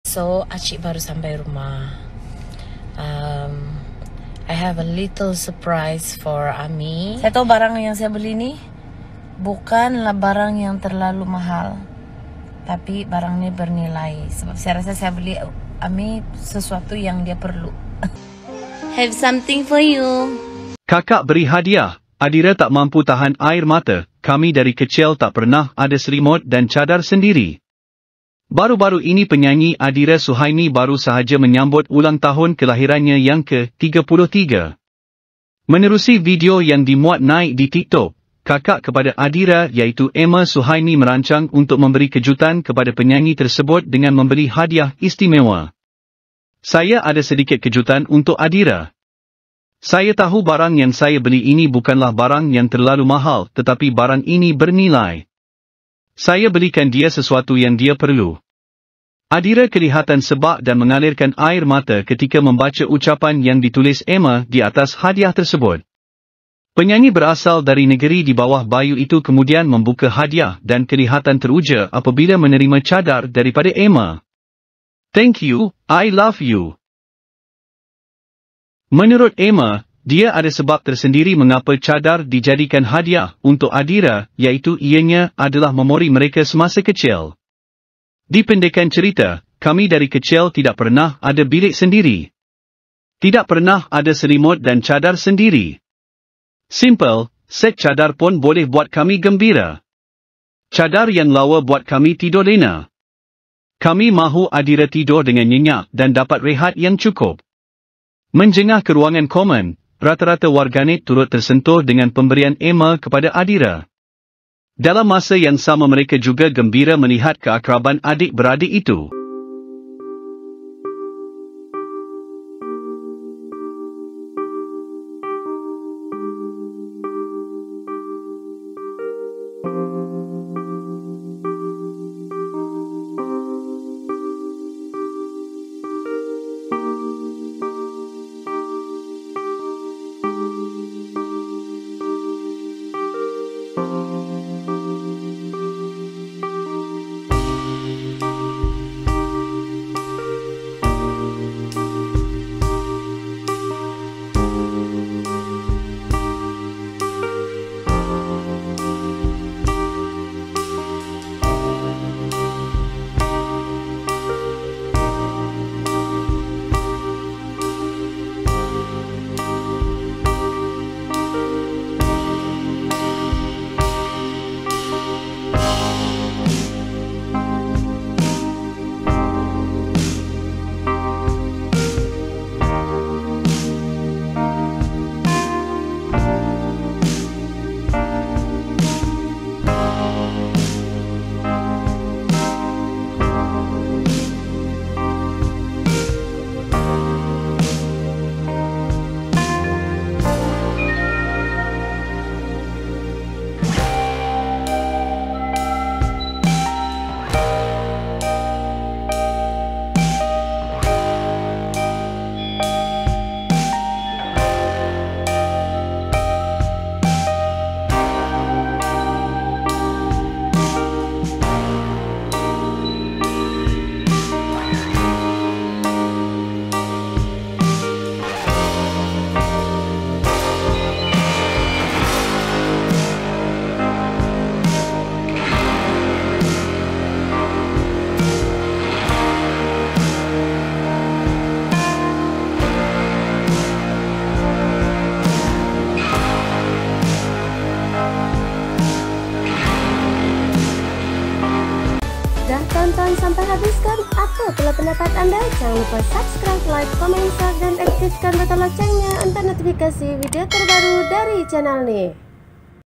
Kakak Beri Hadiah, Adira Tak Mampu Tahan Air Mata
Kakak Beri Hadiah, Adira Tak Mampu Tahan Air Mata – ‘Kami Dari Kecil Tak Pernah Ada Selimut dan Cadar Sendiri’

Sumber :  TikTok Emma Suhaimi , Oh Bulan